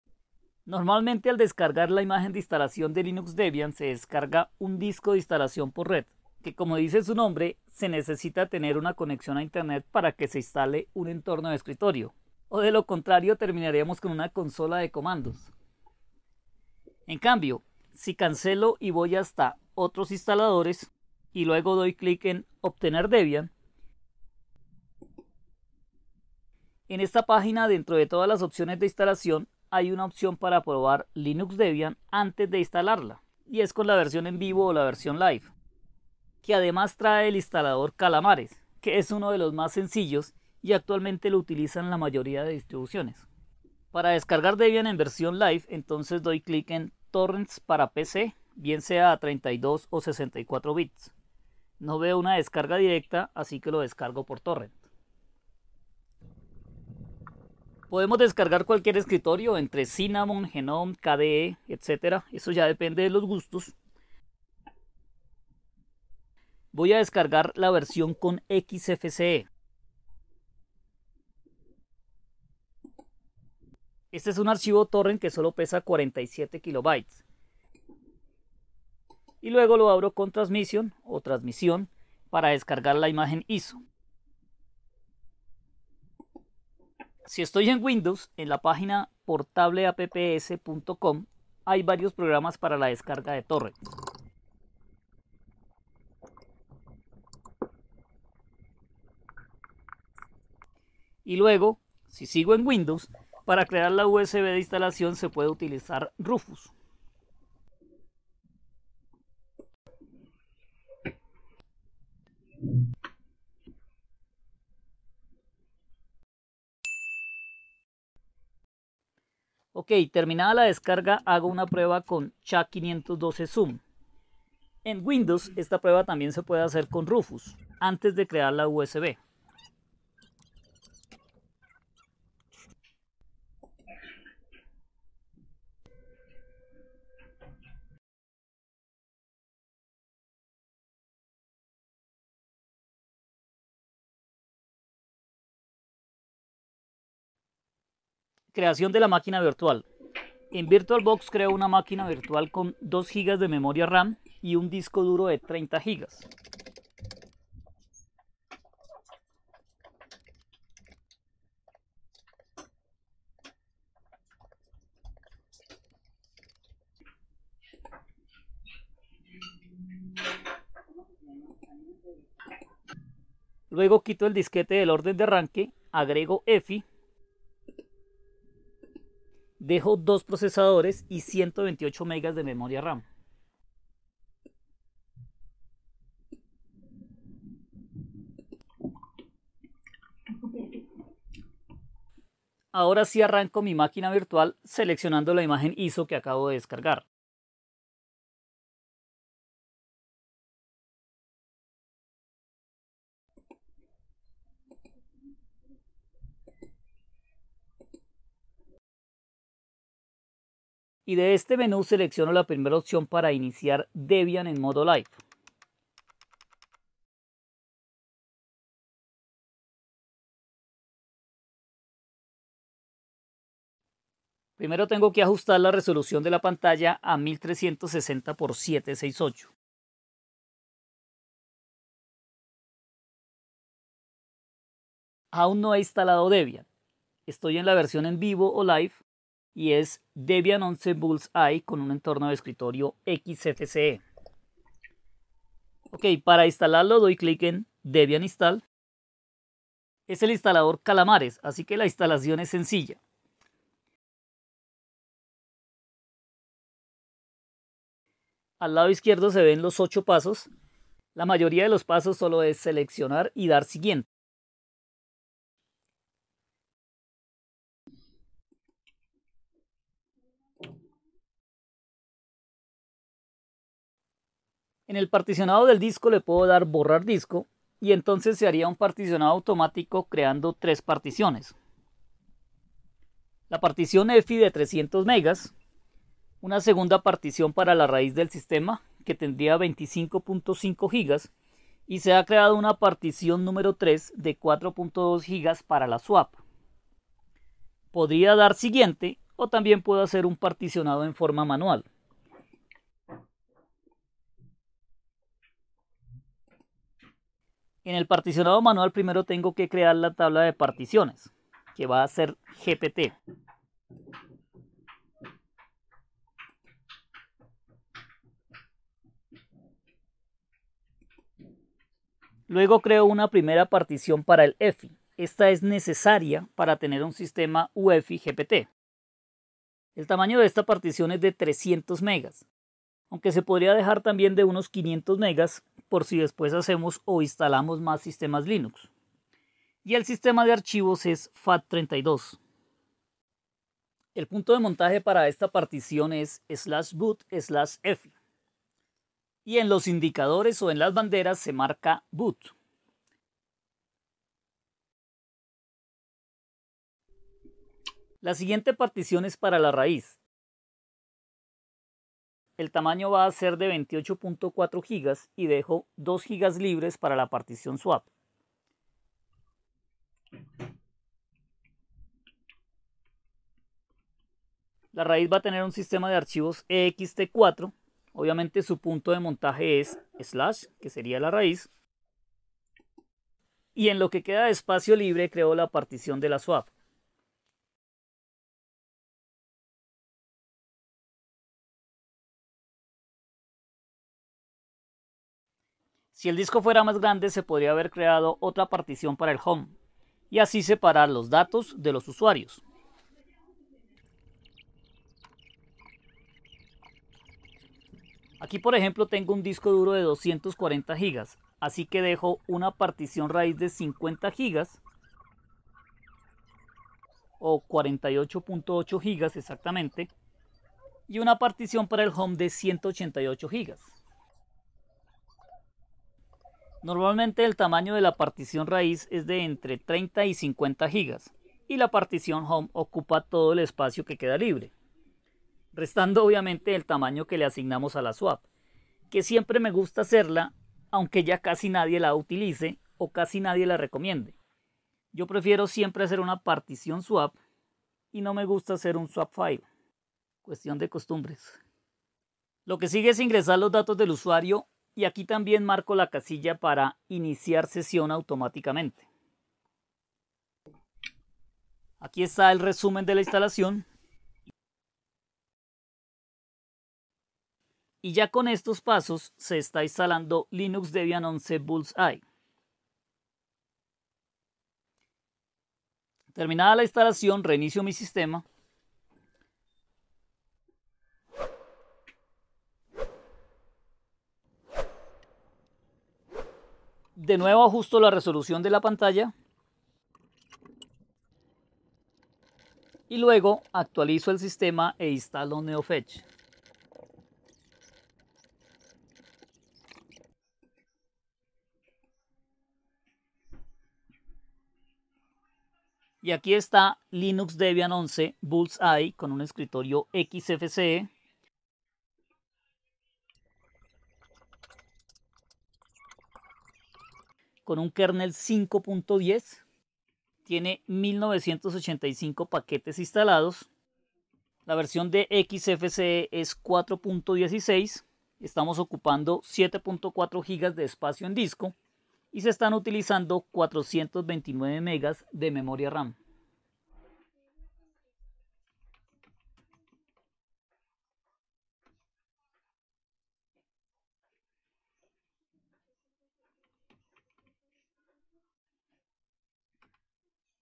Instalar Linux Debian 11 Bullseye 🎯 🐧 Super Fácil con Calamares [V259]
Utilizando la Versión en Vivo o Live, que viene con el Instalador "Calamares", se puede hacer una instalación rápida y fácil del Sistema Debian GNU/Linux.

En el Video se instala la versión con el Escritorio XFCE.  Se explica paso a paso como descargar la imagen ISO de instalación, crear una Máquina Virtual con VirtualBox, lanzar Debian Live y hacer la Instalación con el Instalador Calamares.  Adicional en el paso del particionado del disco se hace tanto el particionado automatico como un particionado manual definiendo las particiones tanto para la raiz /, como para el /home y la partición para la Swap o Memoria de Intercambio.

CONTENIDO / LINEA DE TIEMPO

00:00   Intro - Linux Debian
00:45   Descargar imagen de instalación
02:26   Crear Máquina Virtual
03:10   Iniciar Maquina - Versión En Vivo
04:00   Instalador Calamares
04:58   Particionado manual del Disco
07:55   Reinicio
08:38   neofetch